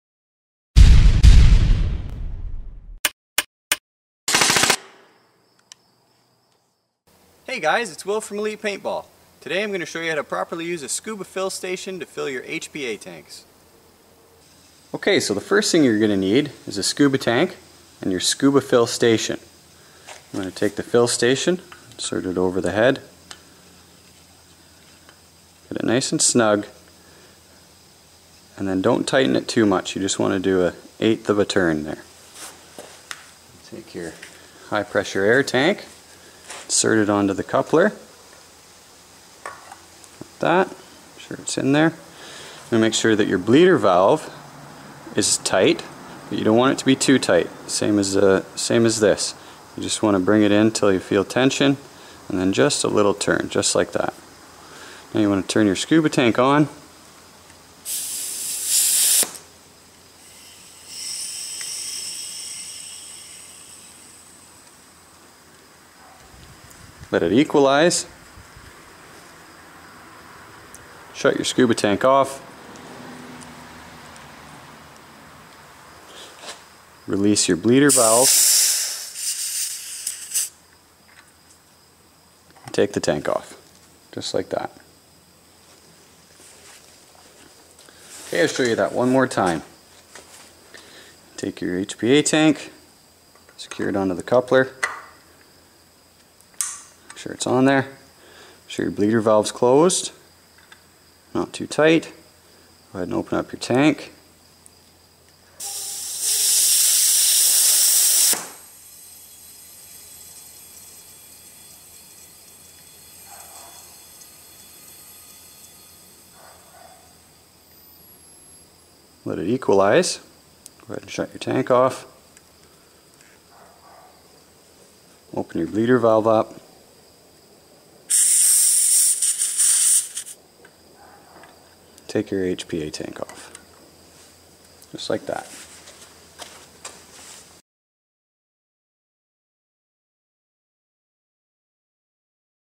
How to use a Scuba Fill Station
Tutorial on how to use a scuba fill station to fill your HPA tanks